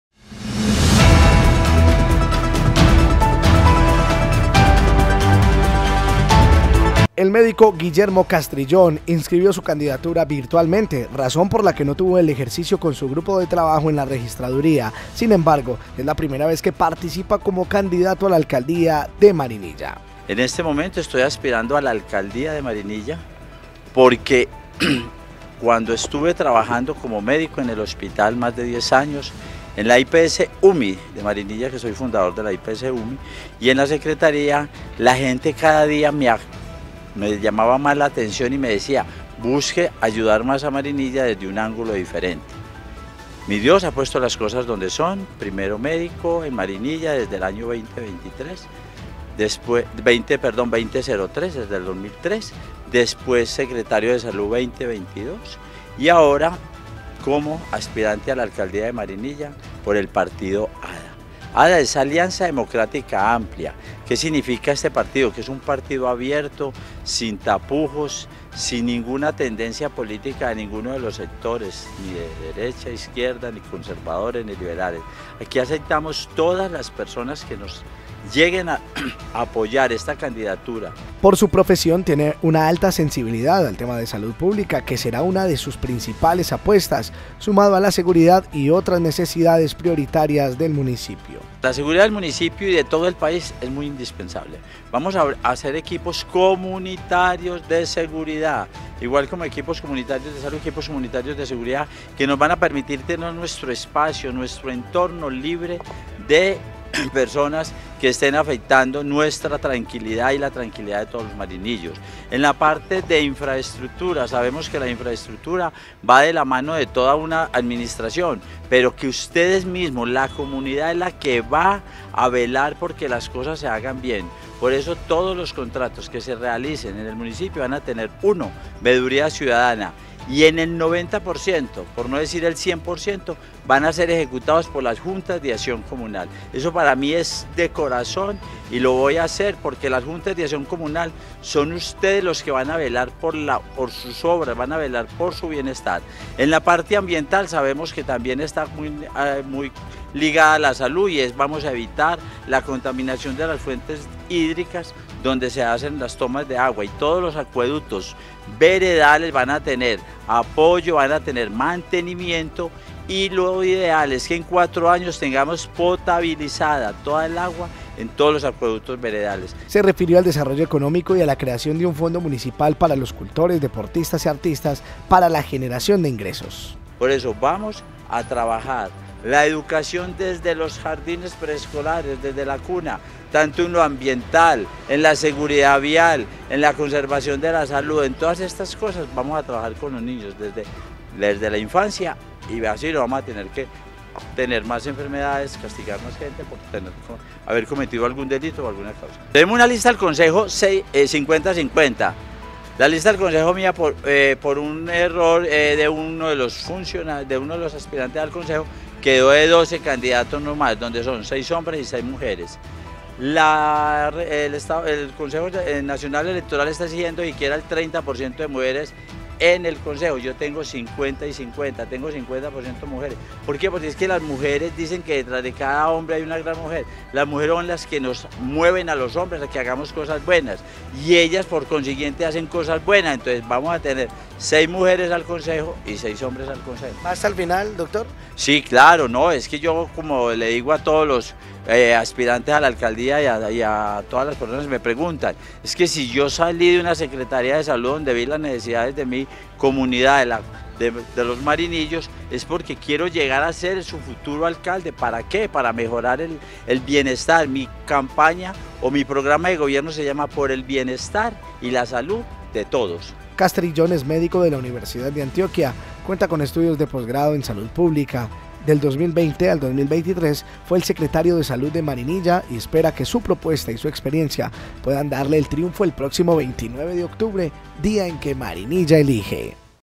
Candidato a la alcaldía Guillermo Castrillón
CCMNoticias| 🗳️ MarinillaElige Son 🔟 los candidatos a la alcaldía de Marinilla. Registramos la inscripción de cada uno de ellos, por eso conoce más sobre la aspiración de Guillermo Castrillón al primer cargo municipal. ⬇️